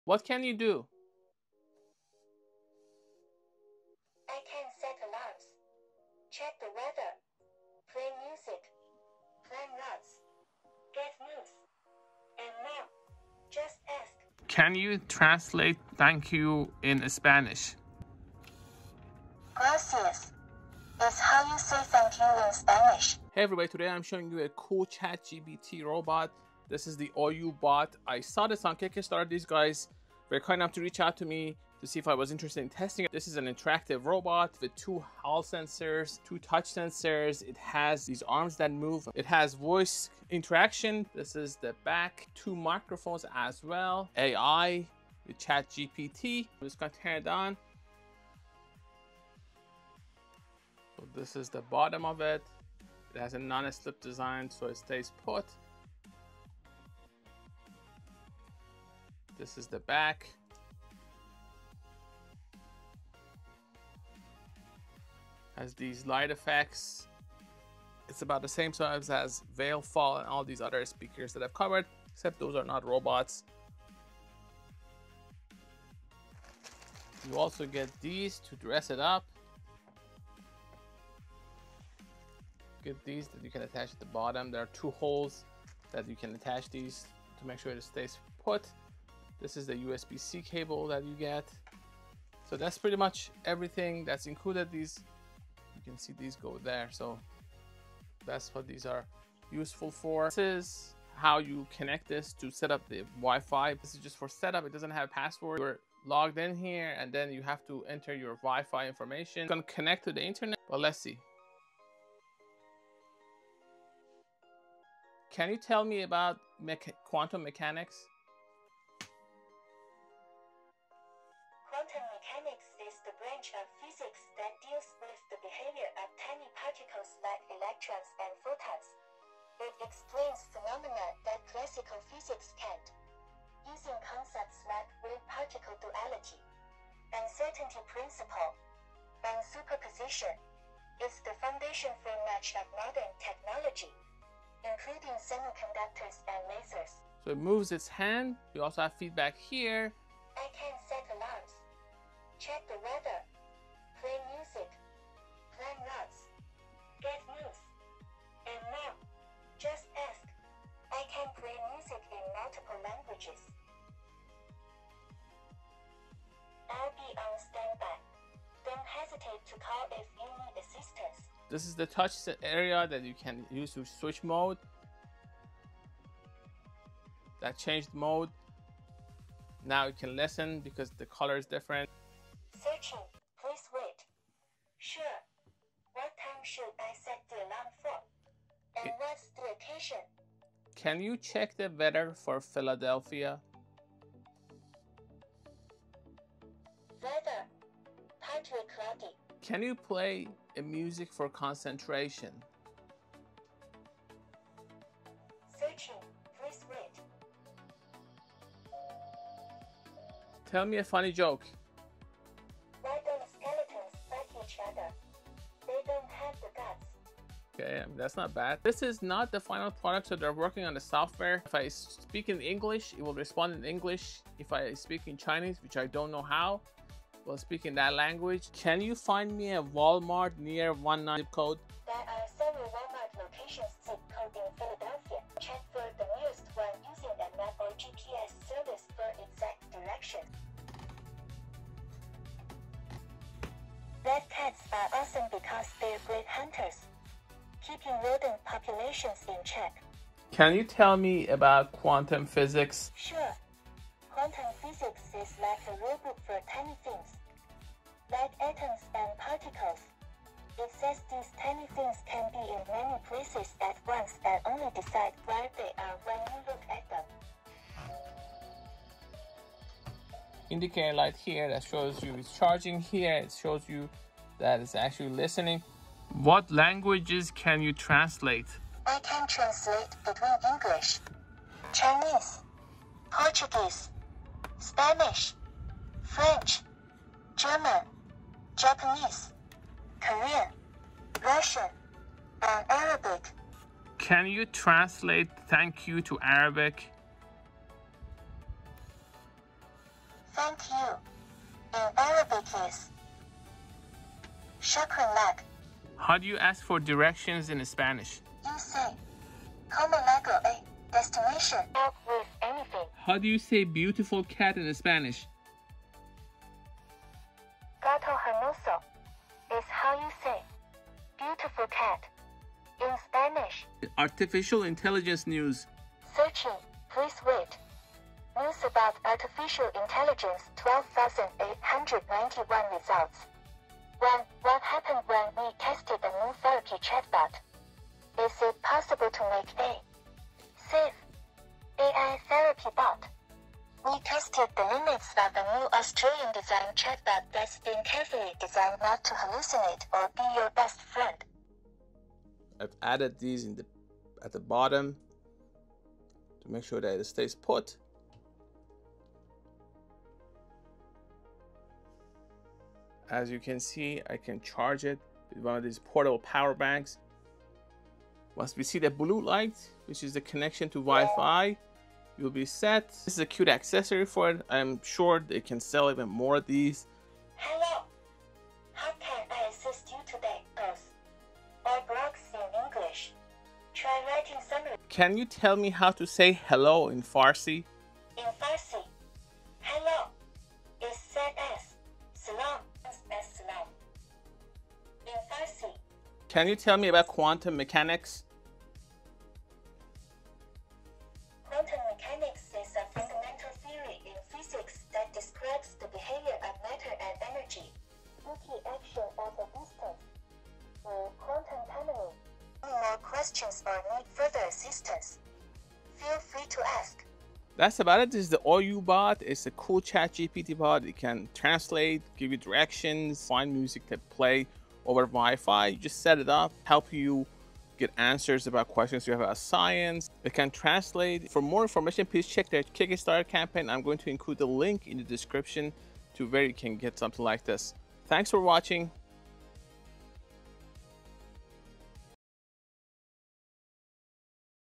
OUROBOT: New ChatGPT Companion Robot Tested 🟡 Gadgetify
ChatGPT and other AI models are becoming a big part of our daily routines. Many of us prefer if they had a physical body. The OUROBOT is designed to do just that. It is a cute desktop companion robot that can answer your questions, translate, check the weather, and find you places around you. It can also act as an educational robot to explain science to your kids. It has motors to move its arms and head. It can also find and play music without a phone.

This robot is on Kickstarter. The good folks behind it sent this to us to test.

*We were given a unit to test.